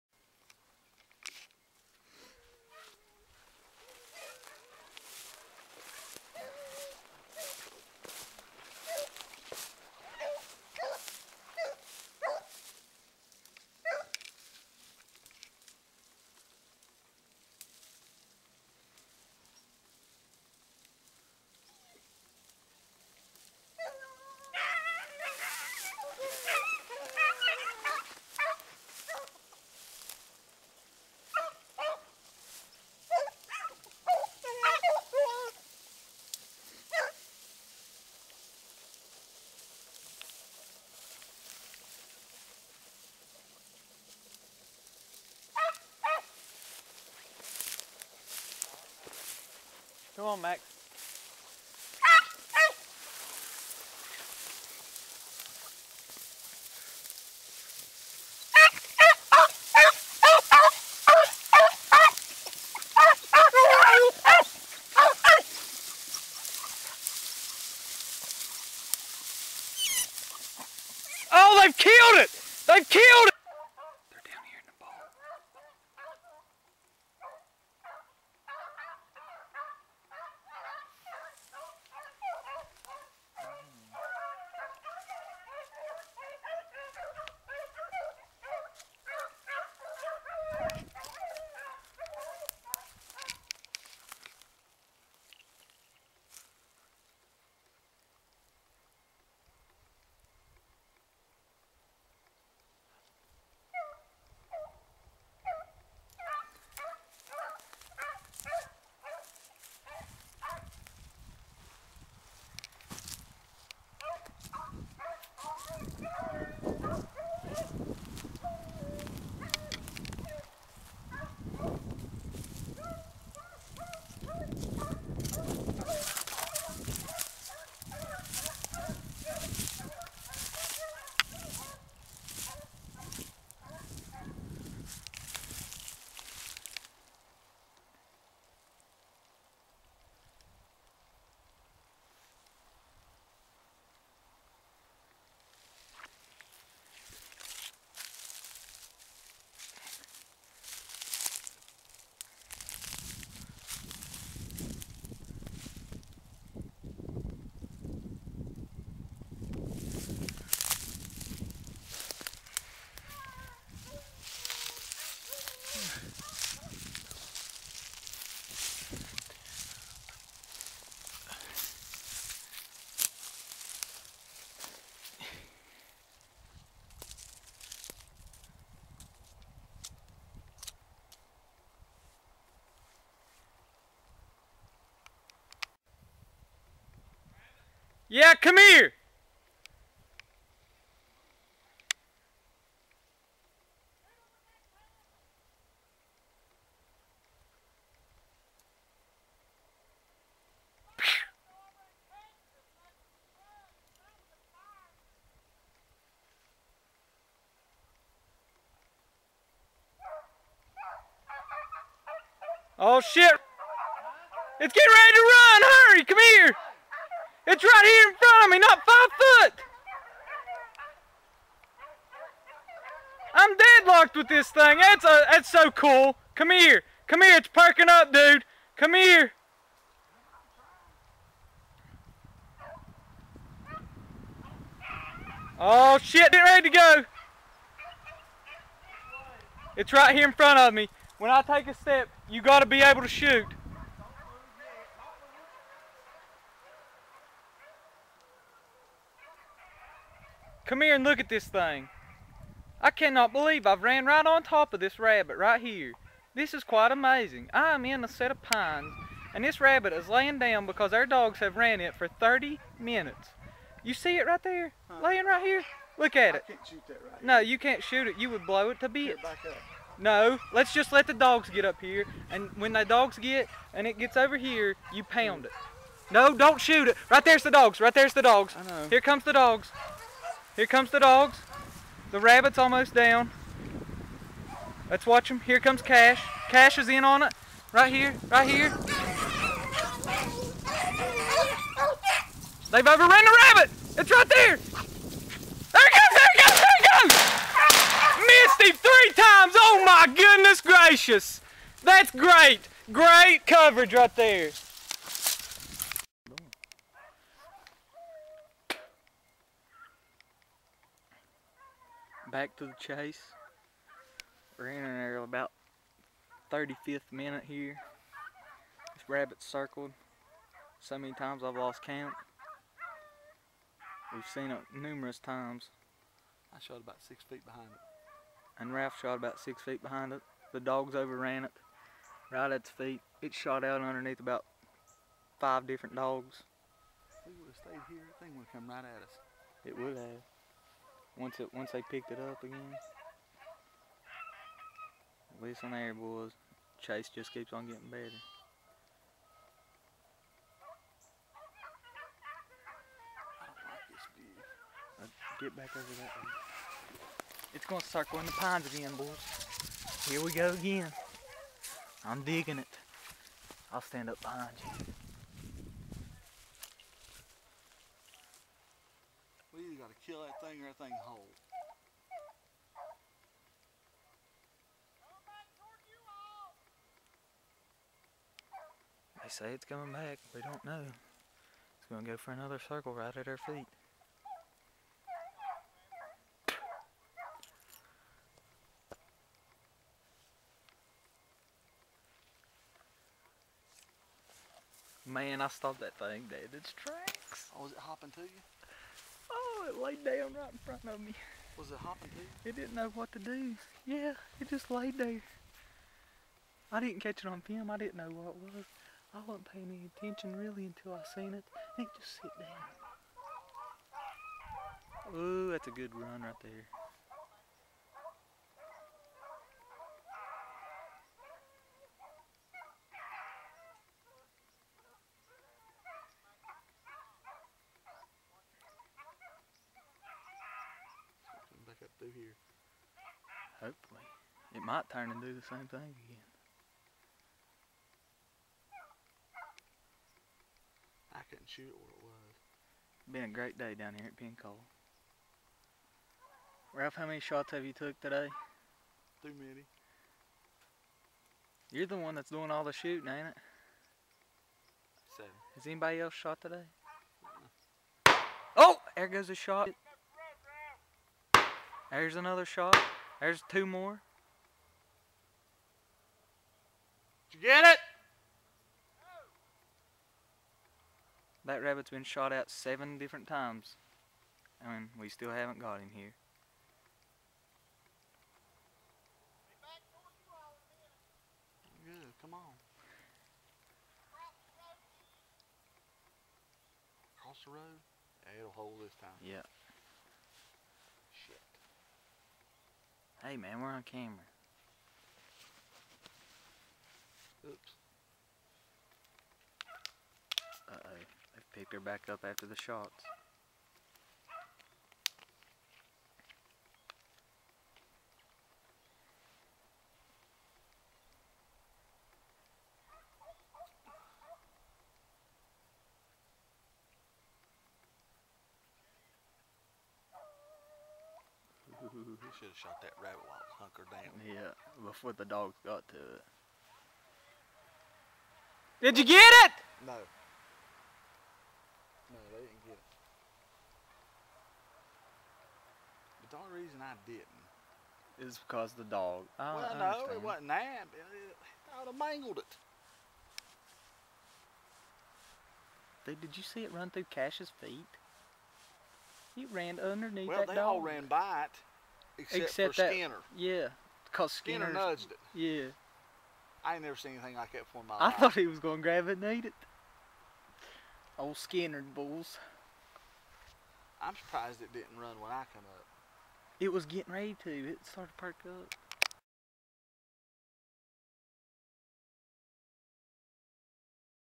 rabbit season 2011-12 another great day
rabbit hunting in wv